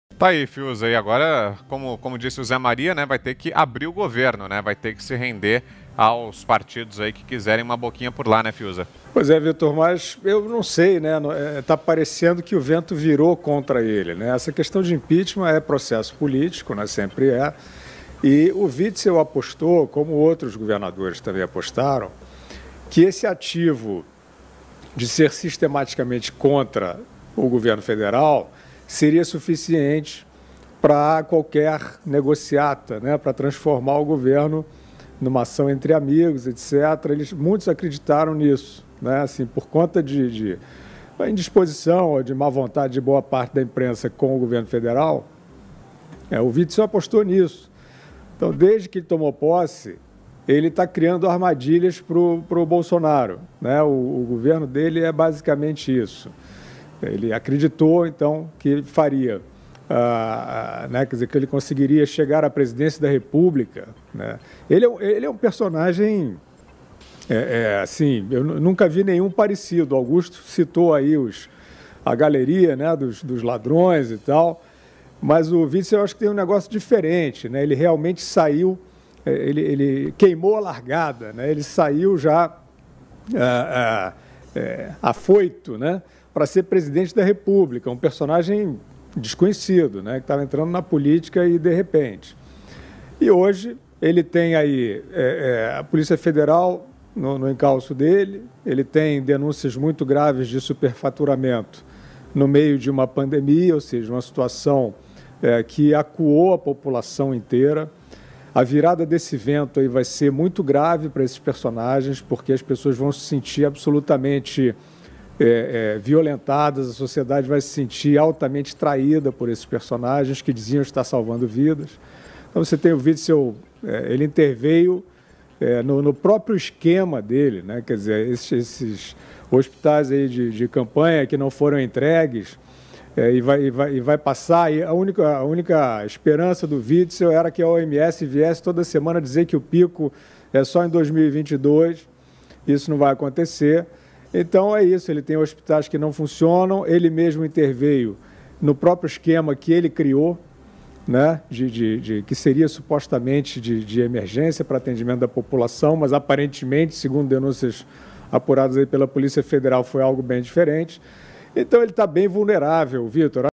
WITZEL É UM PERSONAGEM, FABRICADO E MONTADO POR UM GRUPO OPORTUNISTA.
O problema é que o escroque ganhou inteligência artificial e achou que era maior do que é. Não teve humildade e permaneceu com os mesmo esquemas da velha política.